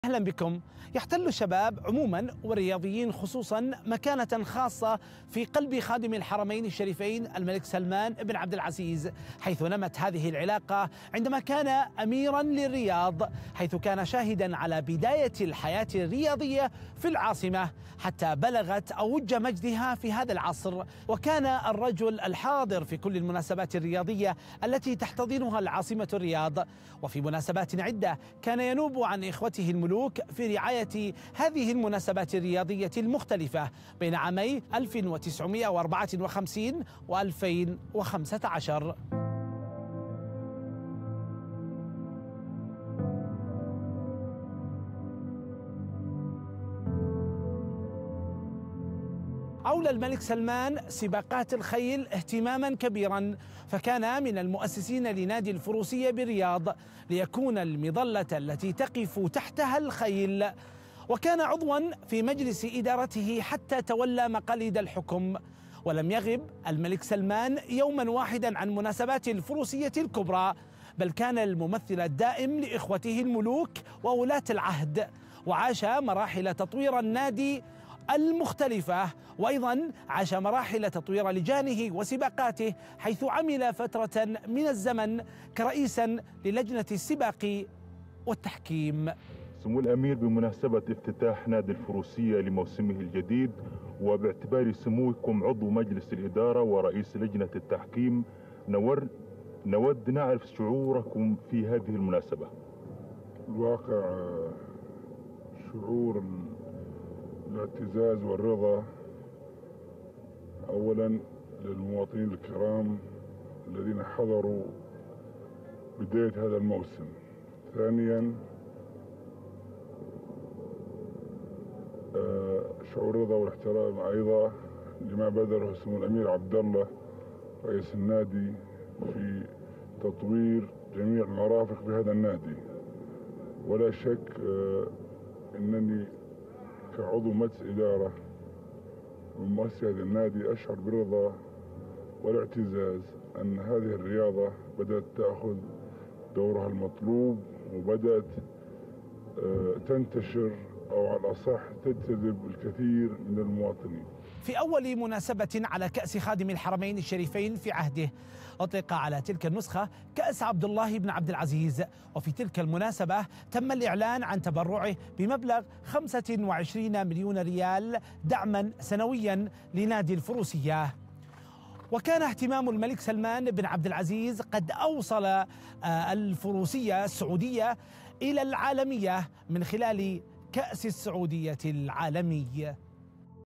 رياضة الفروسية في السعودية.. اهتمام من القادة في المسابقات المحلية والدولية.. التفاصيل مع خلف الخلف
رياضة الفروسية في السعودية.. اهتمام من القادة في المسابقات المحلية والدولية..
التفاصيل مع خلف الخلف